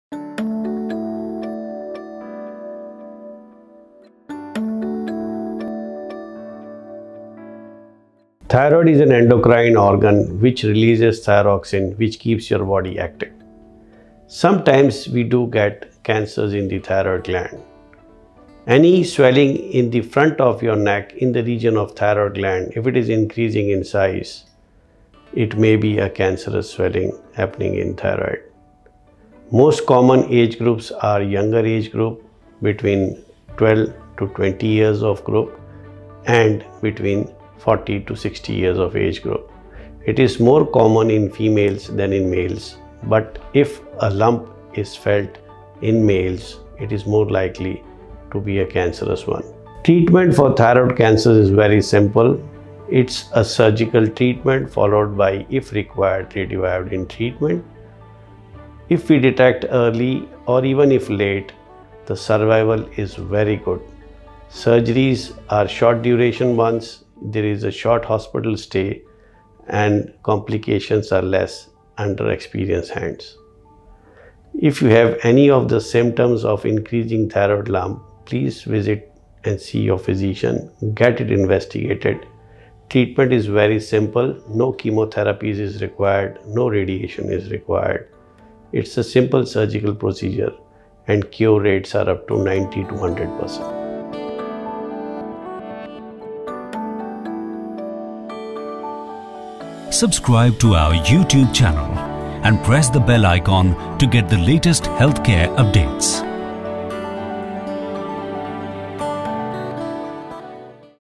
Thyroid Cancer: Diagnosis and Treatment | Dr. Rohit Nayyar | Max Hospital, Gurugram
Thyroid Cancer is most prevalent amongst people of 12 to 20 years and between 40 to 60 years. Director & Clinical Administrator, Surgical Oncology, Max Hospital, Gurugram, explain the diagnosis and treatment of thyroid cancer.